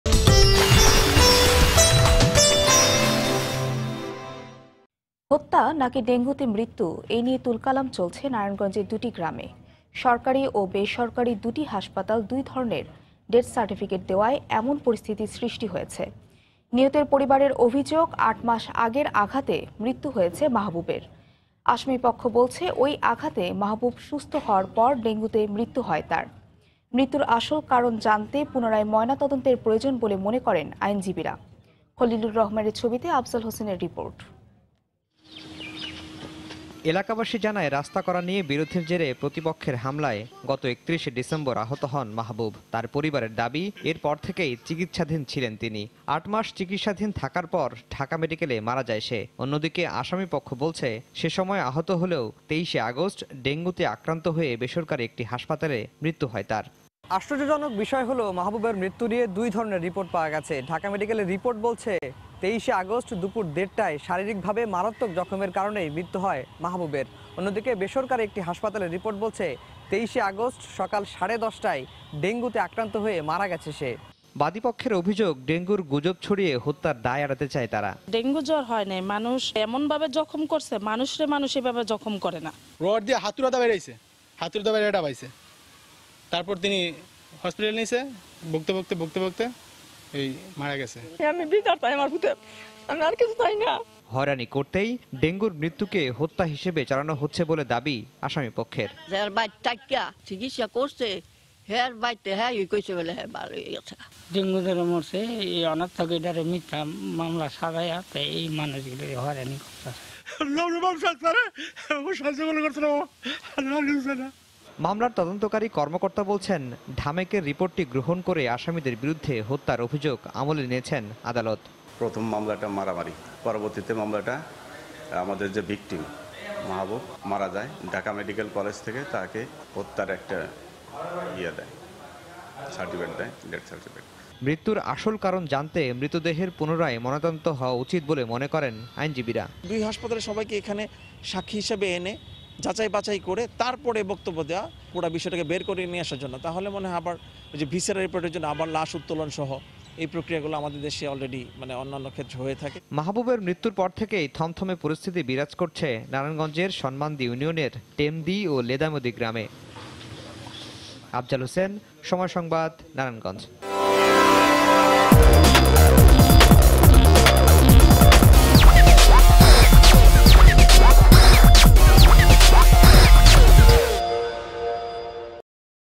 ২ হাসপাতাল ২ ধরণের ডেথ সার্টিফিকেট! | নারায়ণগঞ্জে তুলকালাম! | Somoy TV
হত্যা নাকি ডেঙ্গুতে মৃত্যু এ নিয়ে তুলকালাম চলছে নারায়ণগঞ্জের দুটি গ্রামে। সরকারি ও বেসরকারি দুটি হাসপাতাল দুই ধরণের ডেথ সার্টিফিকেট দেয়ায় এমন পরিস্থিতির সৃষ্টি হয়েছে। নিহতের পরিবারের অভিযোগ, ৮ মাস আগের আঘাতে মৃত্যু হয়েছে মাহবুবের। আসামিপক্ষে বলছে, ঐ আঘাতে মাহবুব সুস্থ হওয়ার পর ডেঙ্গুতে মৃত্যু হয় তার। মৃত্যুর আসল কারণ জানতে পুনরায় ময়না তদন্তের প্রয়োজন বলে মনে করেন আইনজীবীরা। খলিলুর রহমানের ছবিতে আফজাল হোসেনের রিপোর্ট।